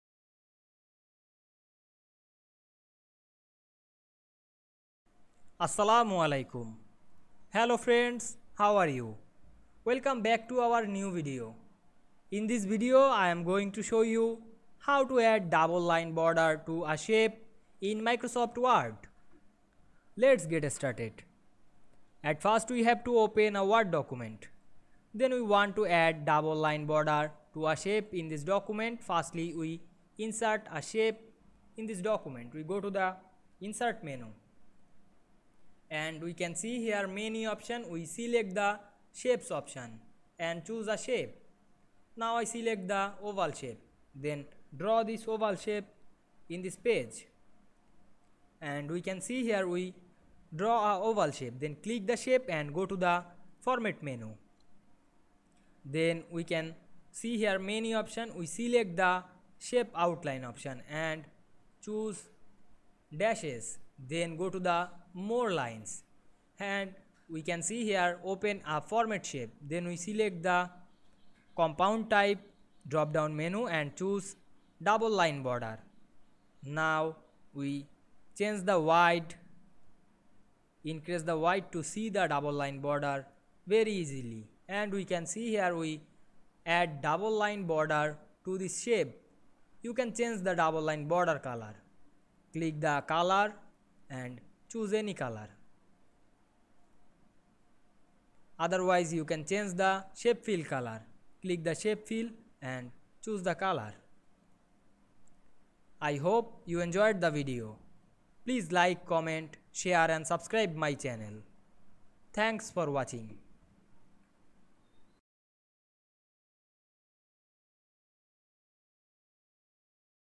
How to add double line border to shape in word
Assalamu Walaikum,
In this video I will show you, How to add double line border to shape in word. Let's get started.